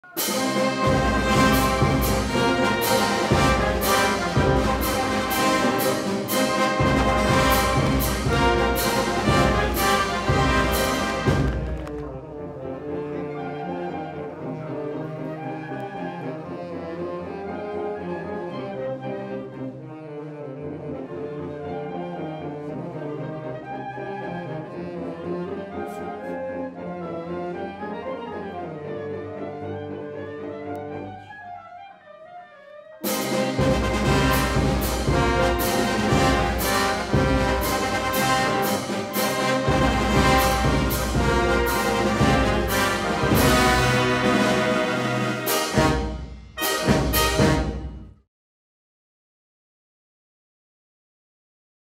Diretta Santi Medici 2019 +++ PROMO +++
Il 20 ottobre, dalle ore 8:00, vivi l'emozione in streaming su e sulla pagina Facebook Radio Doppio Zero. Lo streaming sarà disponibile anche sulla pagina Facebook del Da Bitonto, partner del progetto.